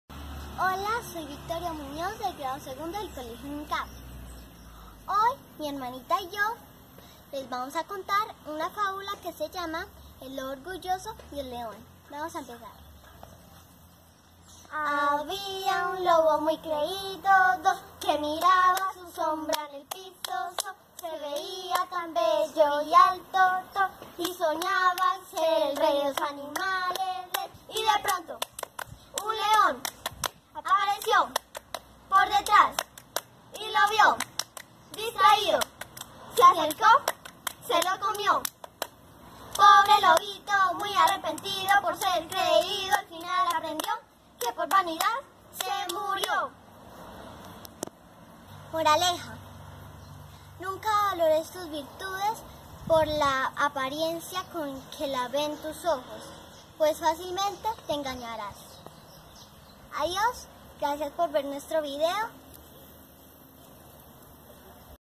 Victoria Muñoz - Fábula el Lobo Orgulloso y el León
Victoria en compañía de su hermana Penélope hacen un divertido juego de palmas donde relatan La Fábula del Lobo Orgulloso y El León.